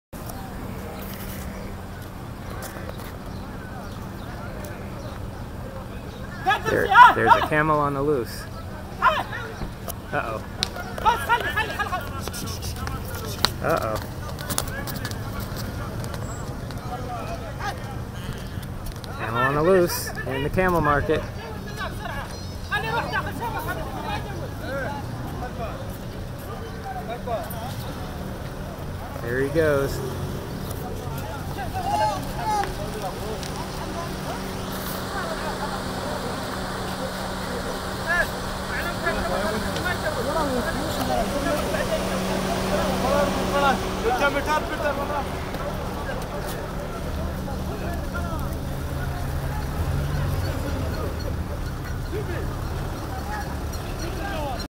Runaway Camel at Camel Market
I took this video at a camel market in Al Ain, UAE.  Considering this market specializes in racing camels, the vendors seem pretty unprepared for what ends up happening...